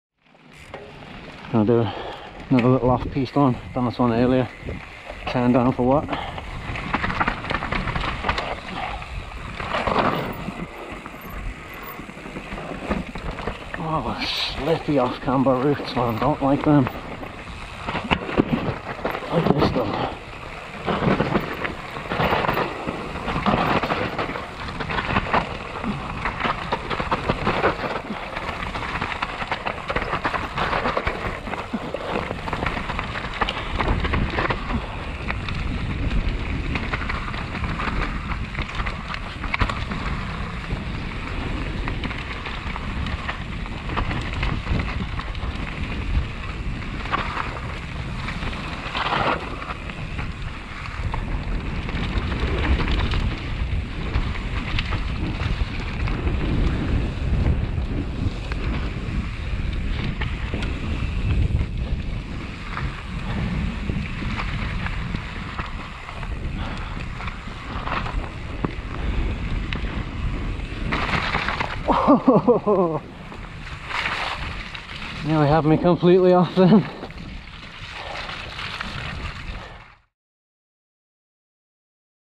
Ae Forest Mtb // Turn Down For What // Adrenalin Uplift
Track Preview for “Turn down for what” at Ae Forest.
This Channel is dedicated to Bringing you along on all my rides plus giving you the Track previews for all the trails!